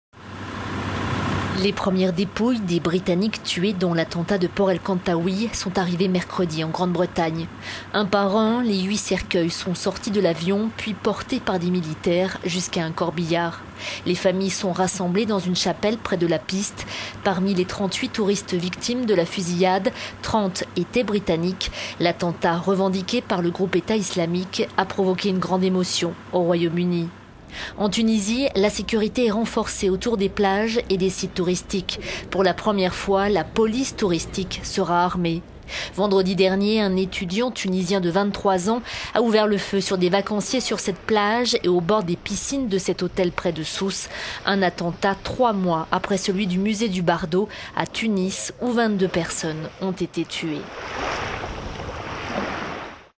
30 Britanniques parmi les victimes de l'attentat en Tunisie
Les premières dépouilles des Britanniques tués dans l'attentat de Port El Kantaoui sont arrivées mercredi en Grande-Bretagne.
Un par un, les huit cercueils sont sortis de l'avion puis portés par des militaires jusqu'à un corbillard. Les familles sont rassemblées dans une chapelle près de la piste. Parmi les 38 touristes victimes de la fusillade, 30 étaient Britanniques. L'attentat, revendiqué par le groupe État islamique, a provoqué une grande émotion au Royaume-Uni.